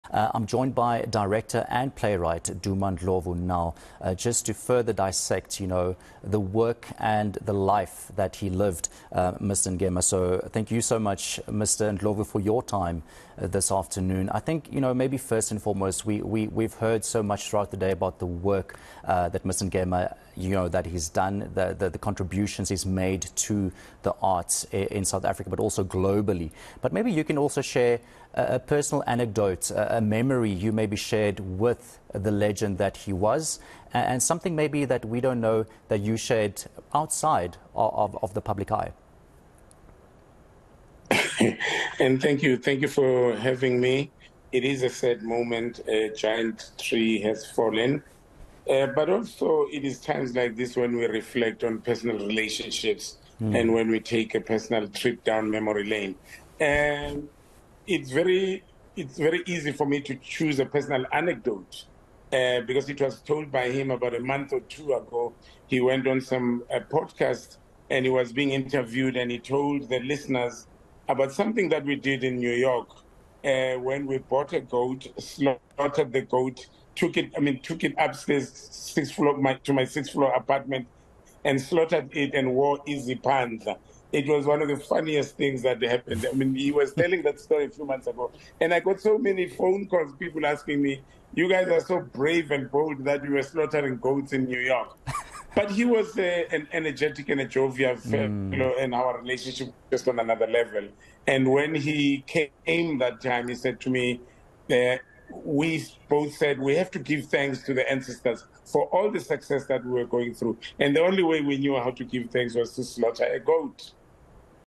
Late Mbongeni Ngema described as a brave freedom fighter by Duma Ndlovu
Renowned filmmaker, Duma Ndlovu says he will remember the late Mbongeni Ngema as a brave freedom fighter who stared the apartheid regime in the eyes and lived to tell a tale.